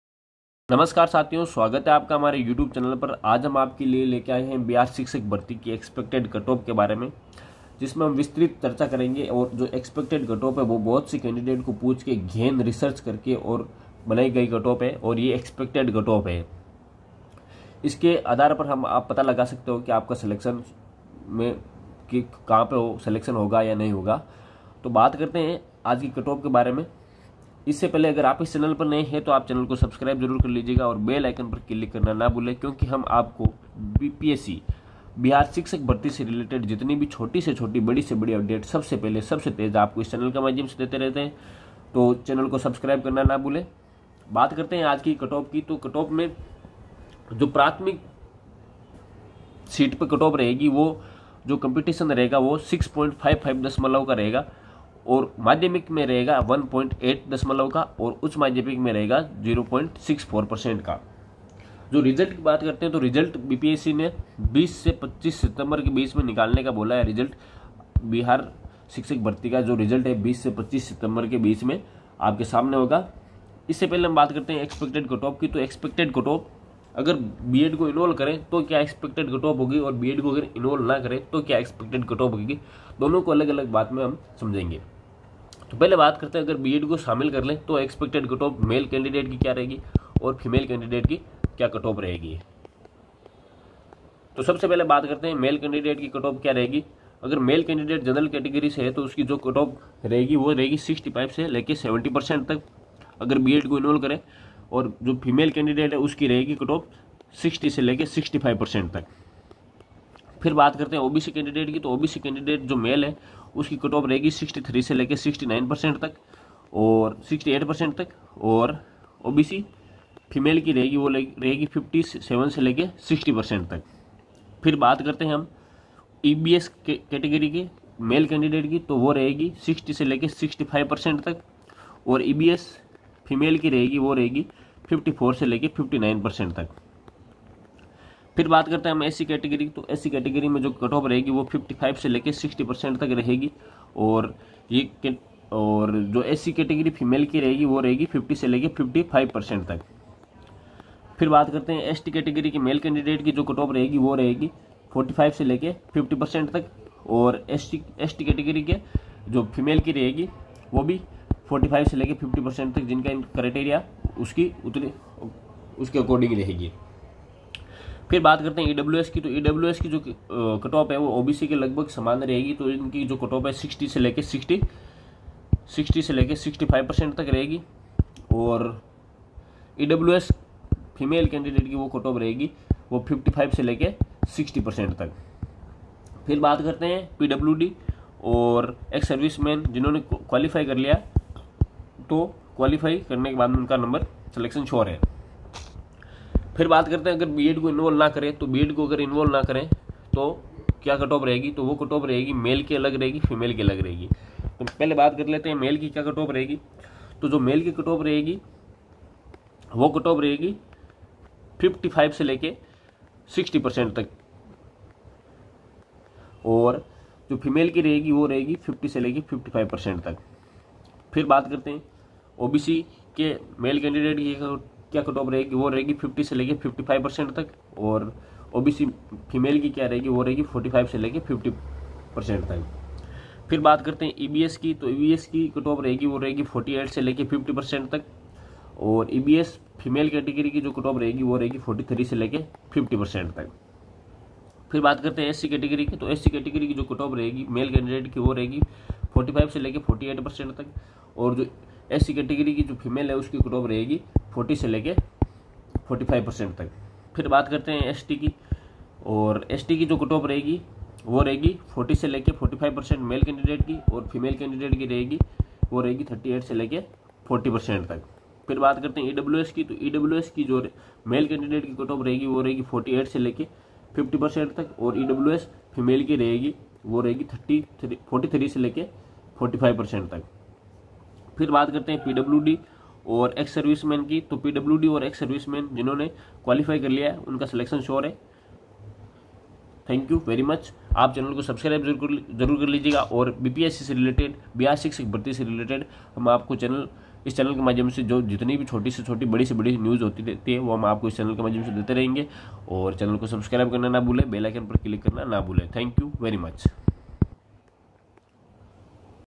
BPSC Teacher Cut off 2023 | बिहार शिक्षक भर्ती Cut-off 2023 | सबसे सटीक CUT-OFF | BPSC Update
नमस्कार साथियों स्वागत है आपका हमारे youtube channel पर
इस वीडियो में हम आपको BPSC TEACHER CUT-OFF 2023 के बारे में संपूर्ण जानकारी दी है